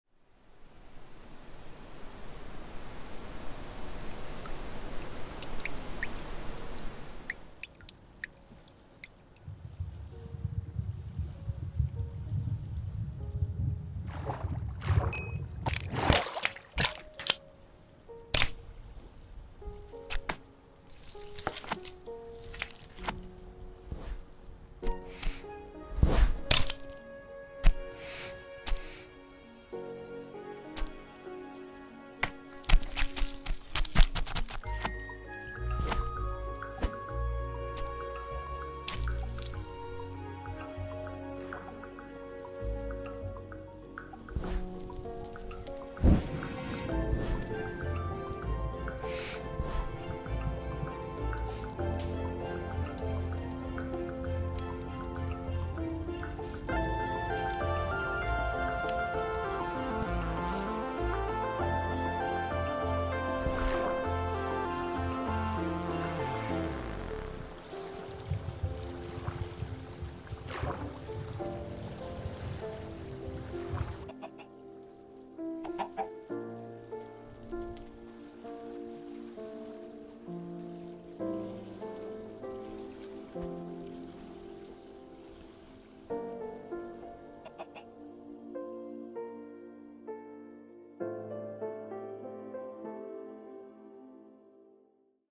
Cooked Fish | Minecraft Cooking Animation
Apologies for the sudden disappearance. School is back, so I had to work on some tasks for a bit. Unfortunately, this animation got delayed, but now I've finished adding the sounds!

After that rainy night, Steve will be cooking fish🐟!